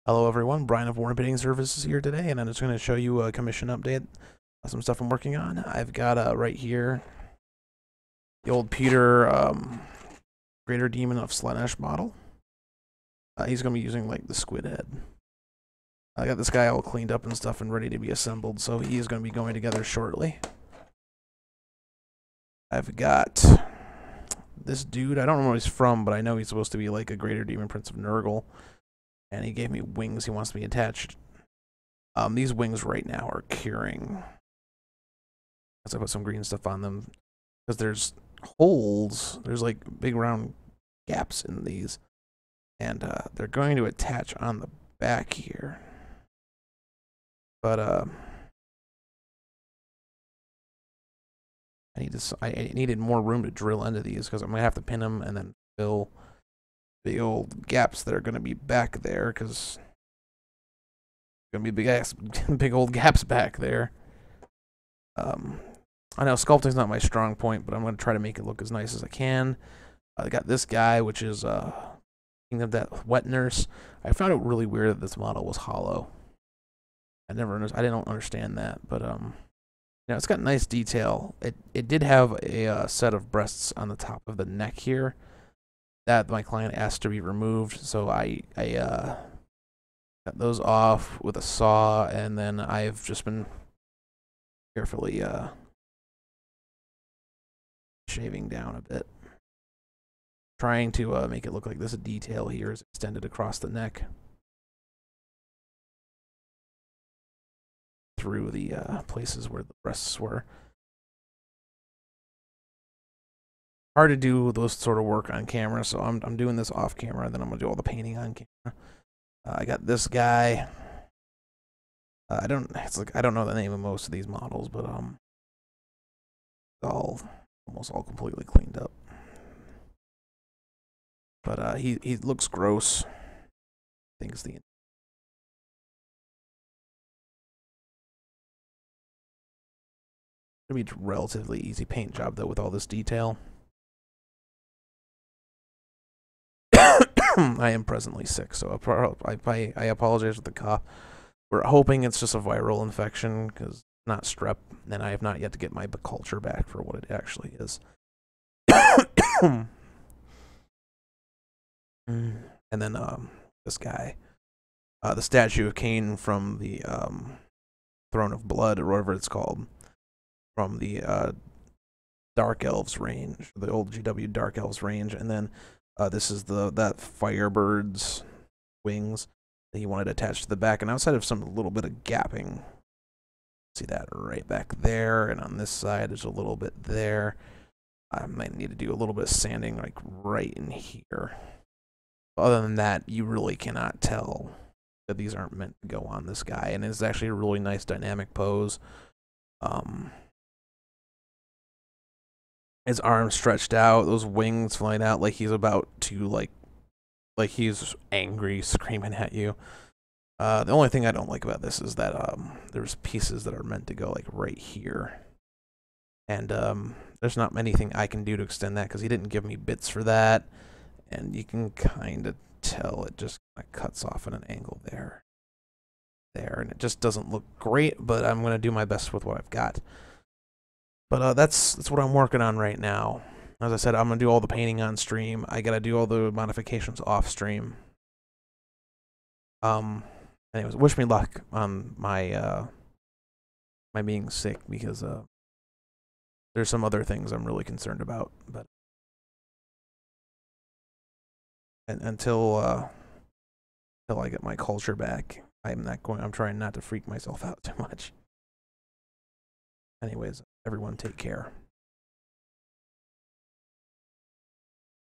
Studio Update June 14th 2016
Five models I'm working on modifying and assembling for a commission right now.